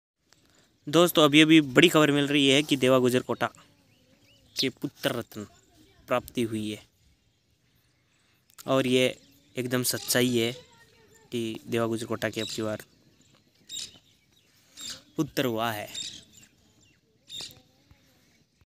deva Gurjar Kota।के हुआ पुत्र।🤱 भगवान के घर देर है अंधेर नहीं। chota Deva 🤱।
देवा गुर्जर कोटा की हुई पुत्र यह बिल्कुल सच्ची खबर है ।